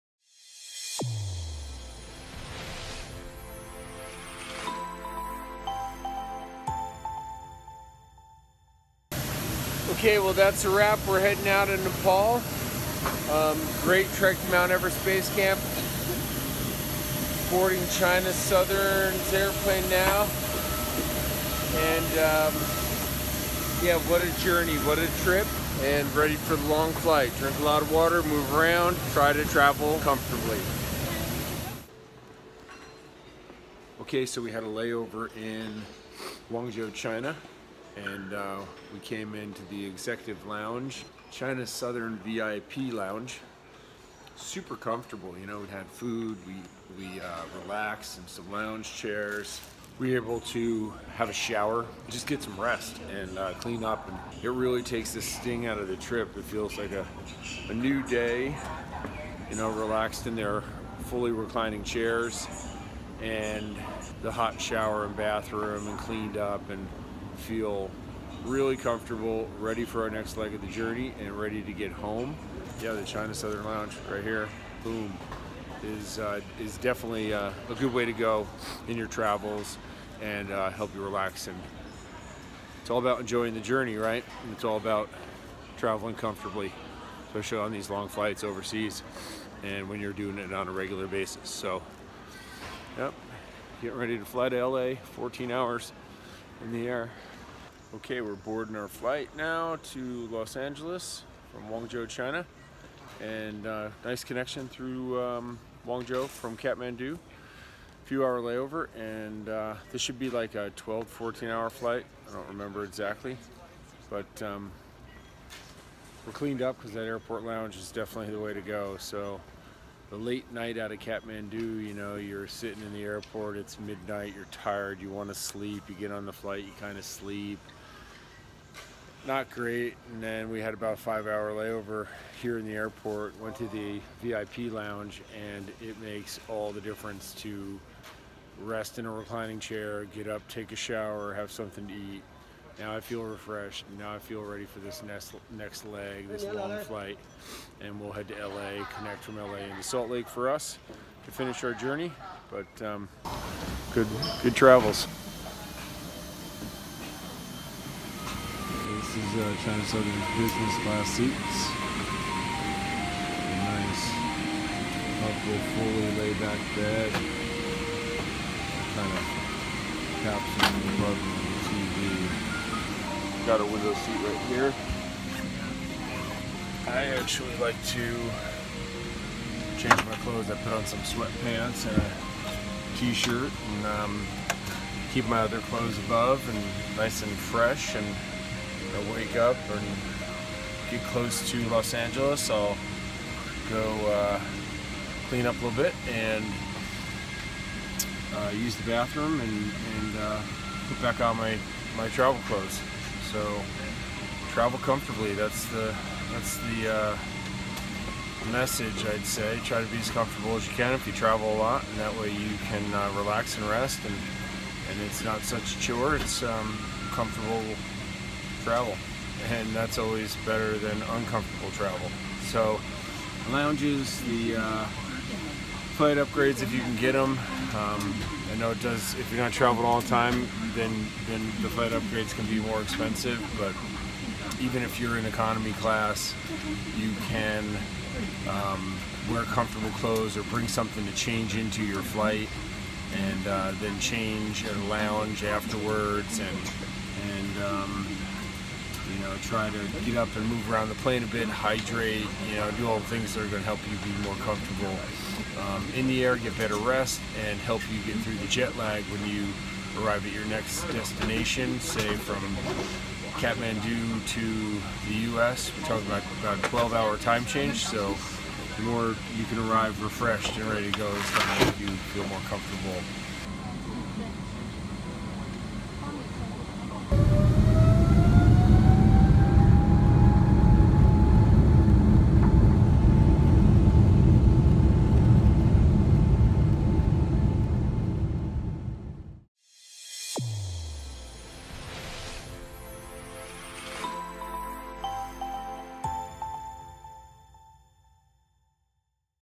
Everest Base Camp Trek - Day 15 - Travel Home from Kathmandu | Trek Tips
➤ Dean departs from Tribhuvan International Airport in Kathmandu, has a layover in Guangzhou, China, and stresses the importance of traveling comfortably! Another successful Everest Base Camp Trek is in the books!